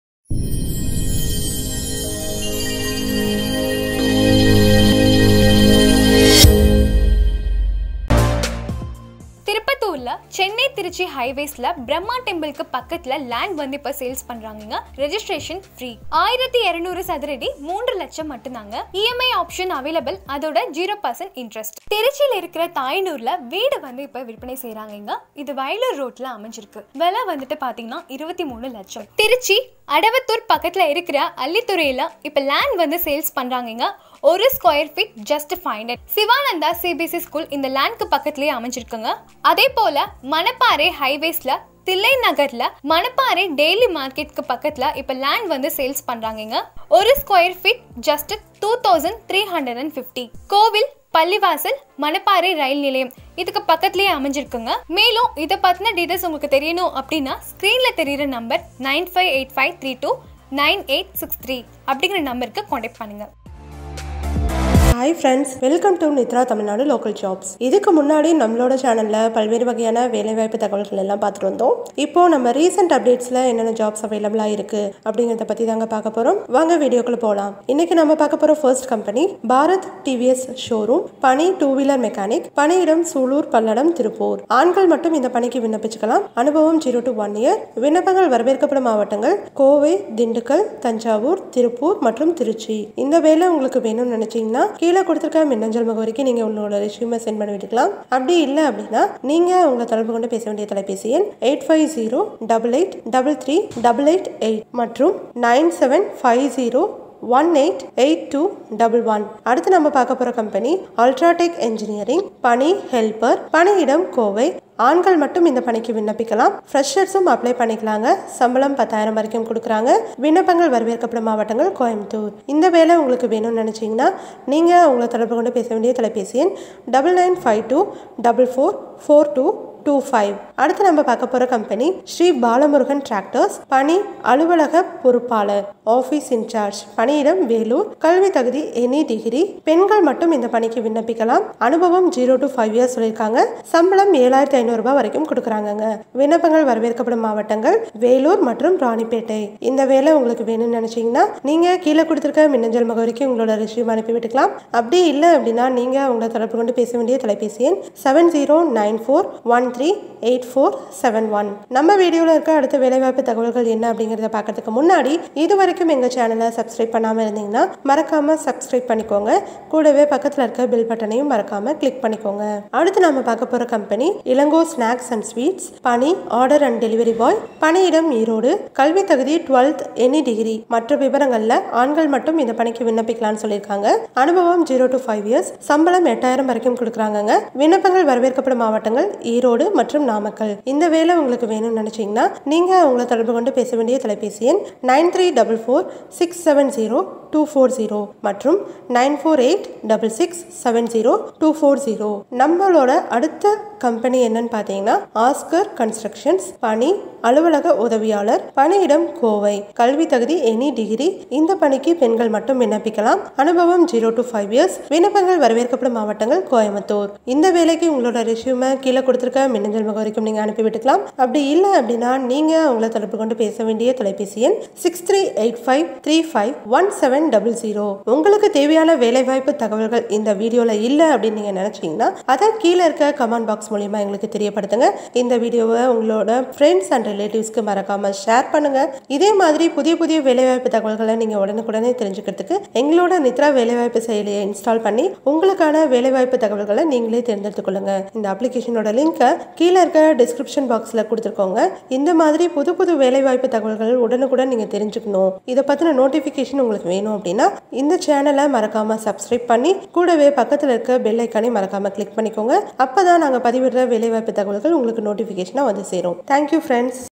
உங்களுக்கான வேலை உங்கள் கையில் ! Any Degree Jobs | Private Jobs 2021 !
1. நிறுவனம் :  Bharath TVS Showroom
பணி : டூ வீலர் மெக்கானிக் (Two Wheeler Mechanic)
 பணியிடம் : சூலூர், பல்லடம், திருப்பூர்

2. நிறுவனம் : Ultra Tech Engineering
 பணி :  உதவியாளர் (Helper)
 பணியிடம் : கோவை

3. நிறுவனம் : Sri Balamurugan Tractors (Eicher)
 பணி : அலுவலக பொறுப்பாளர் (Office Incharge)
 பணியிடம் : வேலூர்

4. நிறுவனம் : Elango Snacks & Sweets
 பணி : Order & Delivery Boy)
 பணியிடம் : ஈரோடு

5. நிறுவனம் : Oscar Constructions
 பணி : அலுவலக உதவியாளர் (Office Assistant)
 பணியிடம் : கோவை